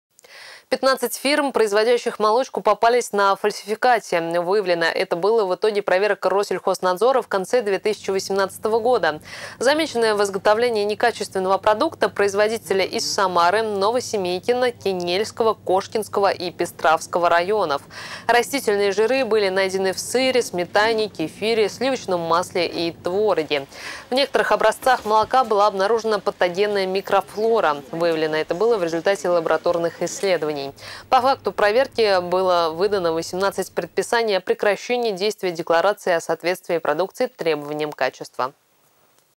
15 фирм, производящих "молочку", попались на фальсификате в Самарской области
Выявлено это было в итоге проверок Россельходнадзора в конце 2018 года. Замечены в изготовлении некачественного продукта производители из Самары, Новосемейкино, Кинельского, Кошкинского и Пестравского районов. Растительные жиры были найдены в сыре, сметане, кефире, сливочном масле и твороге. В некоторых образцах молока была обнаружена патогенная микрофлора. Ее выявили в результате лабораторных исследований. По факту проверки было выдано 18 предписаний о прекращении действия деклараций о соответствии продукции требованиям качества.